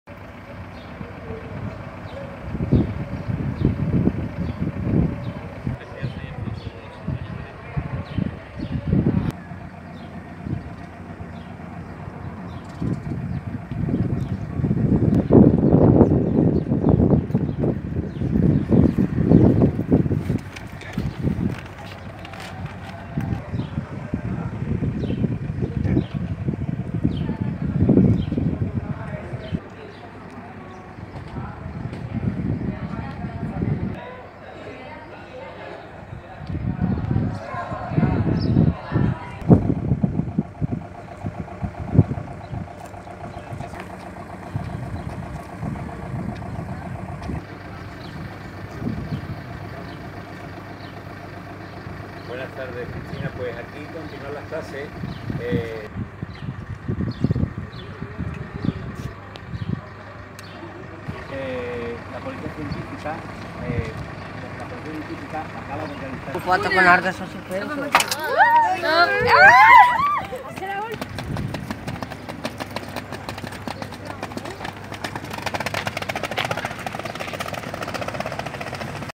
Tres heridos con quemaduras en una deflagración en un instituto de Sevilla
Dos alumnos menores de edad y una profesora han sido trasladados al Hospital Universitario Virgen del Rocío de Sevilla al resultar heridos con quemaduras en una deflagración registrada en la mañana de este miércoles un instituto de Bellavista, en Sevilla capital.